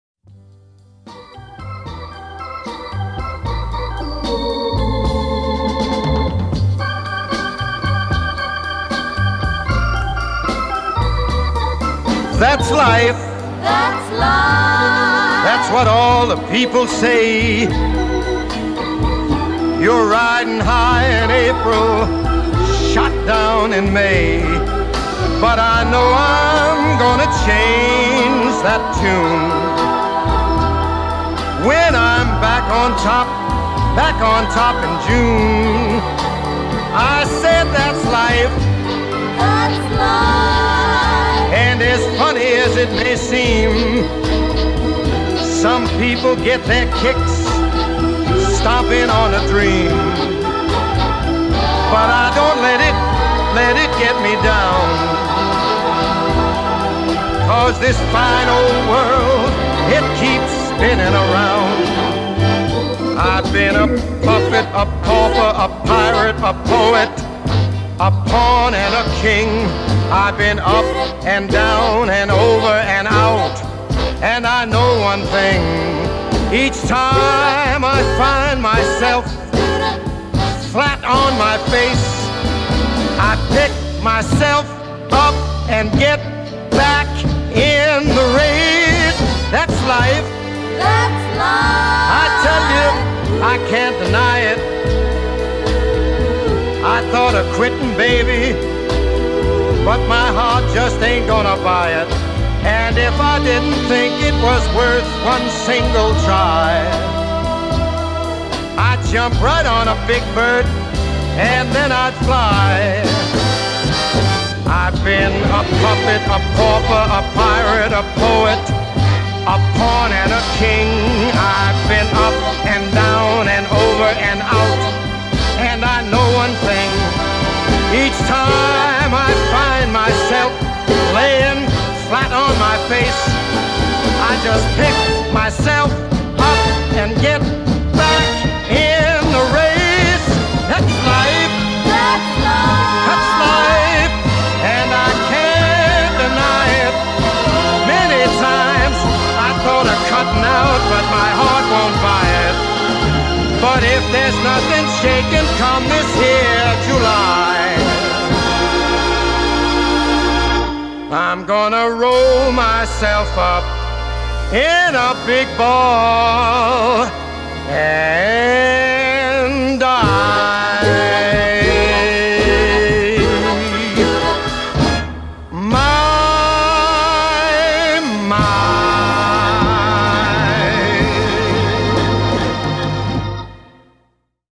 Drawing Joker's Face(Ink Attempt)
Ink is hard,but so nice to use!!!